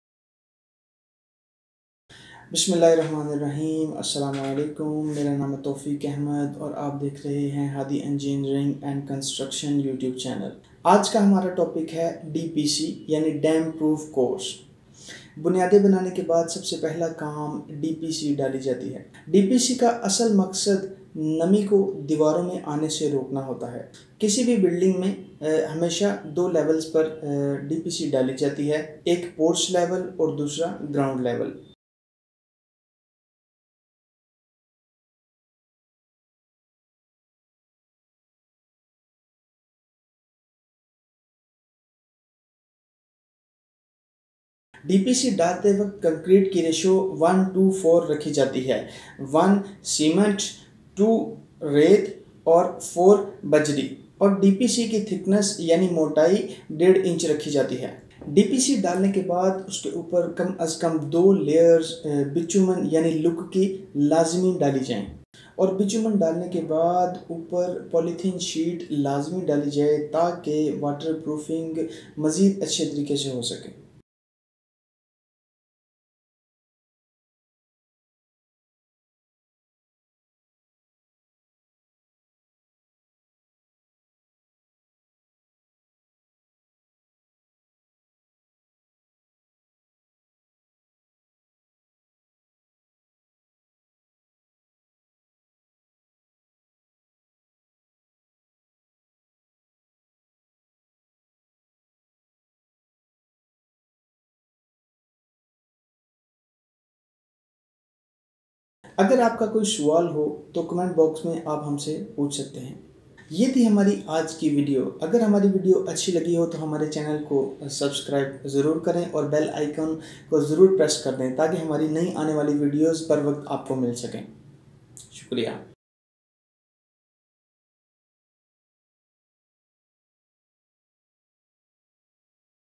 DPC | What Is DPC (Damp Proof Course) ? DPC in Building Construction | HADI Eng & Con.
What is the Damp Proof Course?
In order to prevent the entry of damp or moisture in the building, the damp proof course (D.P.C) is provided at various levels of the entry of damp into the building. ... Thus the provision of damp-proof course prevents the entry of moisture from walls, floors, and basement of a building.
Why do we use DPC?
A damp-proof course is one of the most important elements of a property. The DPC protects the property against moisture rising from the ground